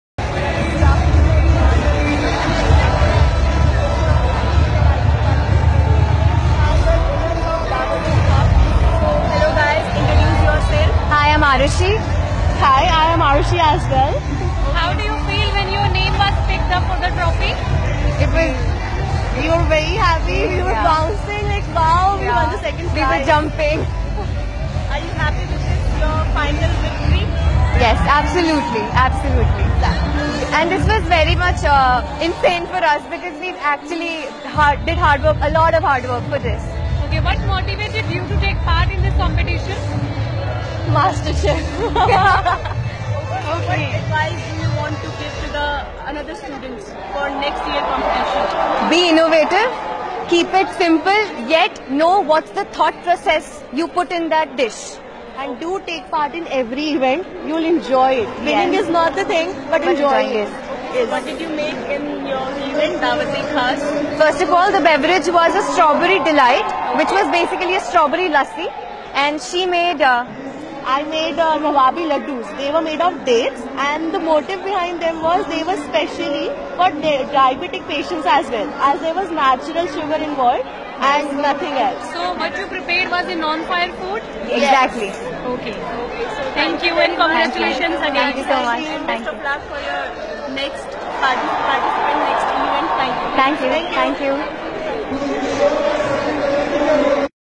CPJCOLLEGE- DIWALI CARNIVAL ULLAAS 2018, INTERVIEW BITE WITH WINNERS OF DAAWAT-E-KHAS
ULLAAS - 2018, A Grand Diwali Carnival! Ms.Kalpana Bhora ,Assistant Professor-IT along with Ms. Nisha Singh ,Assistant Professor-Law interacting with the runner up of DAWAT-E-KHAS @ ULLAAS 2018.